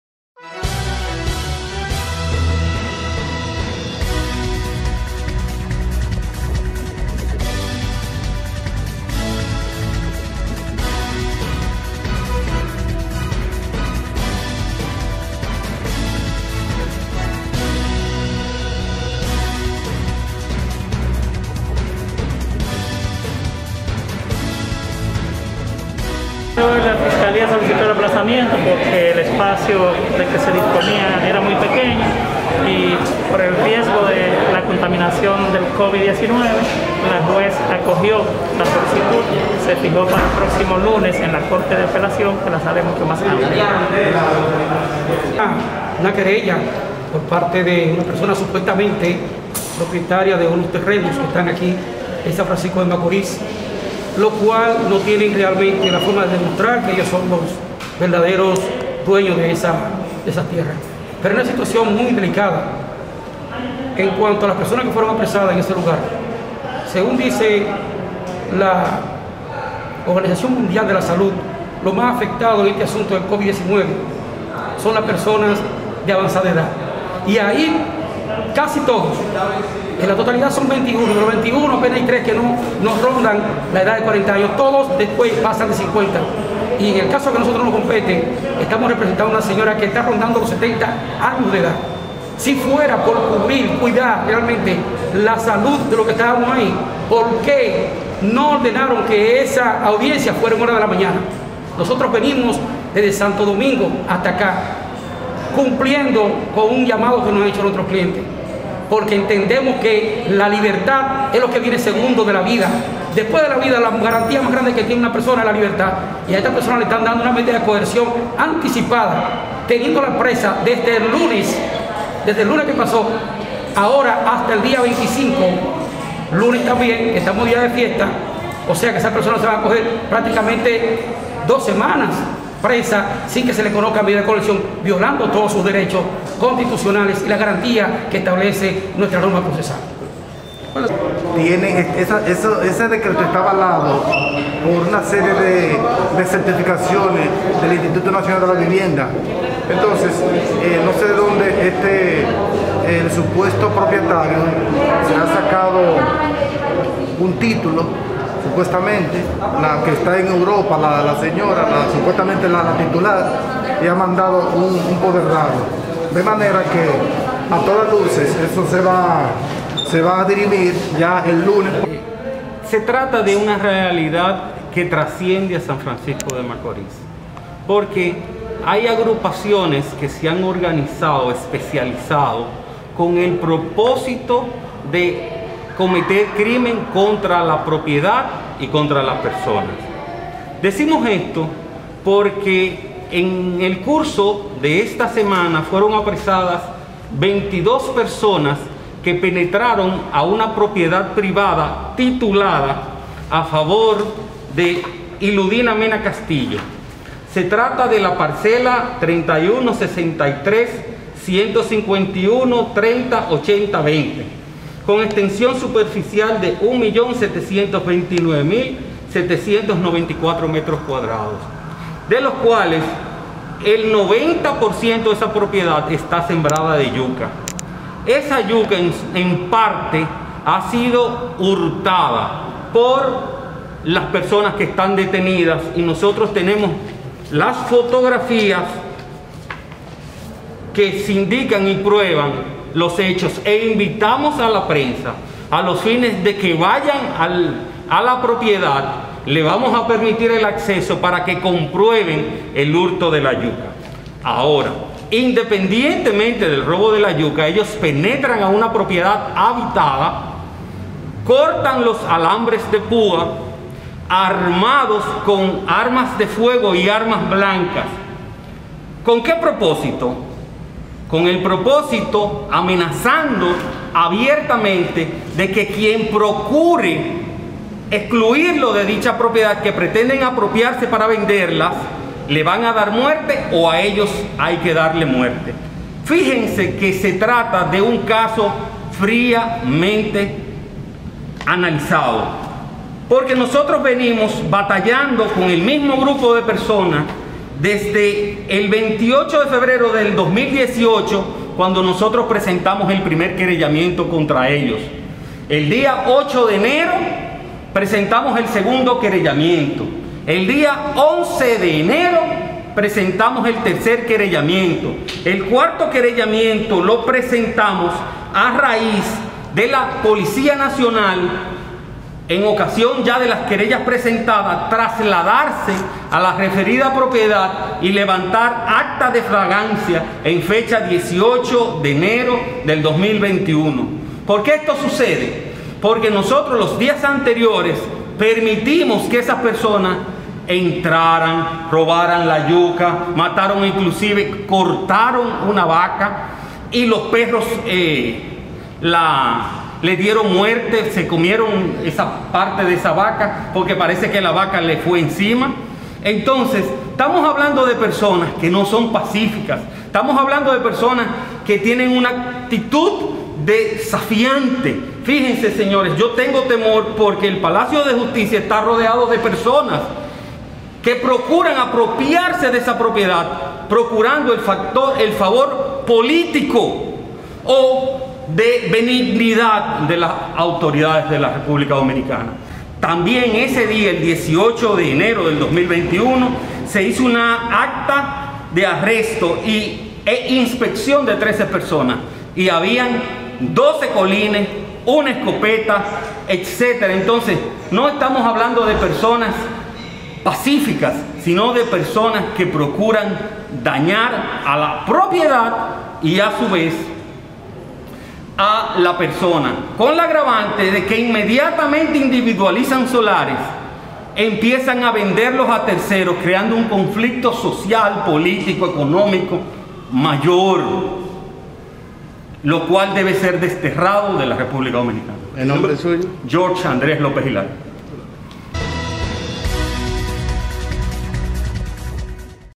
Aplazan conocimiento de medida de coerción contra 21 campesinos en SFM
Fue aplazada para el próximo lunes 25 de enero, el conocimiento de medida de coerción contra 21 campesinos que fueron detenidas invadiendo unos terrenos en la comunidad de Hatillo de San Francisco de Macorís.

El abogado Francisco, dijo que la audiencia fue aplazada para resguardar los protocolos del COVID-19, ya que la sala donde se conocía la medida de coerción es muy pequeña para los 21 detenidos y los abogados que representan a los mismos.

Por su parte los abogados de los detenidos dicen que  a estos se le están violando sus derechos, ya que la gran mayoría de los apresados, sobrepasan los 60 años de edad y son personas vulnerables al COVID-19, incluyendo una señora de 70 años.

Según el abogado que representa a la señora Iludina Mena Castillo, presunta propietaria de la parcela marcada con el número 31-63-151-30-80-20, con un tamaño de 1,729,794.000 metros cuadrados,  los detenidos han hurtado el 90% de la tierra que está sembrada de yuca, así como también han penetrado a ocupar los terrenos de manera amenazante.

Se espera que la audiencia a estos 21 campesinos se reanude este próximo lunes 25 de enero a las 10:00 de la mañana.